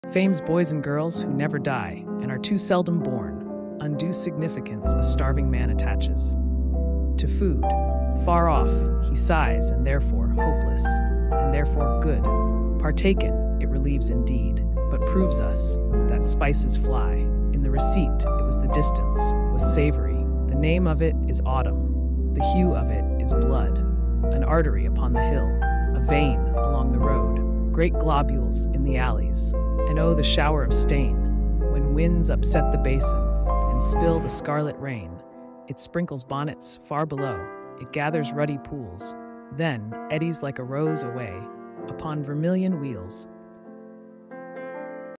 Fame's Boys and Girls, who never die, Undue Significance a starving man attaches, The name
Verselets: Fame's Boys and Girls, who never die, Undue Significance a starving man attaches, The name -- of it -- is "Autumn" --, Emily Dickinson

Paintings from the Art Institute of Chicago:
1. The Indestructibles, Philip Evergood
American, 1901–1973

2. Jesus Mocked by the Soldiers, Édouard Manet
French, 1832–1883

3. The Wedding at Cana, Giuseppe Maria Crespi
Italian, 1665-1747

4. Red and Pink Rocks and Teeth, Georgia O'Keeffe (American, 1887–1986)

5. Two Sisters (On the Terrace), Pierre-Auguste Renoir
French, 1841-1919

6. Self-Portrait, Vincent van Gogh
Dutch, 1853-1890

7. The Interior of the Palm House on the Pfaueninsel Near Potsdam, Carl Blechen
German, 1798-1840

8. Fish (Still Life), Édouard Manet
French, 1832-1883

9. Red Yellow Blue White and Black II, Ellsworth Kelly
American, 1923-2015

10. Fisherman's Cottage, Harald Sohlberg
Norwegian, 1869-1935

11. Stacks of Wheat (End of Summer), Claude Monet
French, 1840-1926

12. The Resurrection, Francesco Buoneri, called Cecco del Caravaggio
Italian, 1588/90–after 1620

13. Into the World There Came a Soul Called Ida, Ivan Albright (American, 1897–1983)